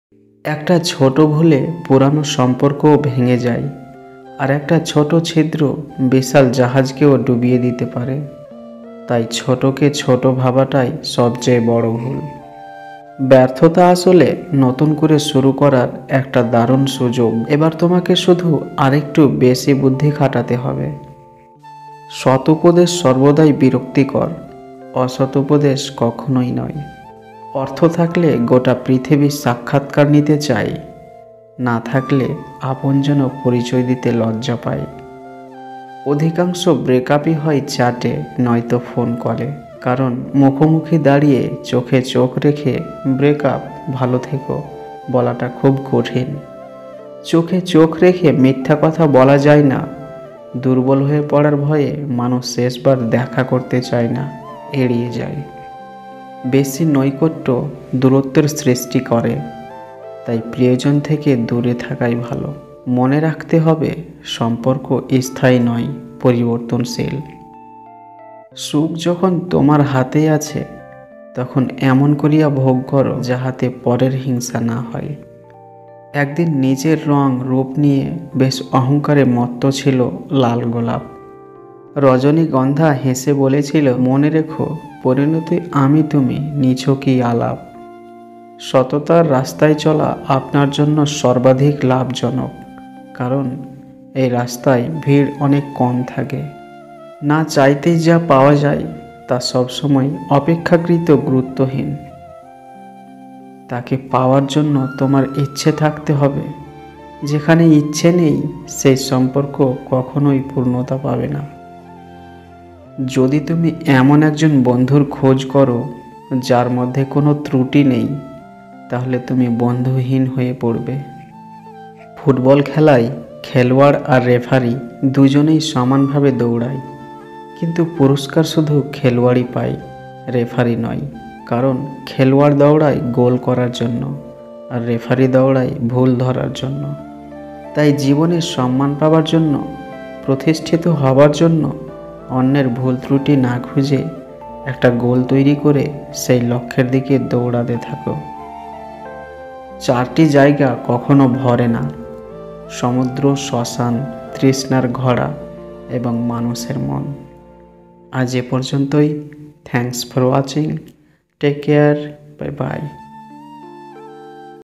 Life changing heart touching motivational quotes in Bangla|| Love status|| Bani|| ukti
Heart touching short story and motivational quotes in bangla that will change your thought process & change your life surely.These quotes are of successful great men of the world like APJ Abdul Kalam, Shakespeare, Chanakya, Billgates and so on.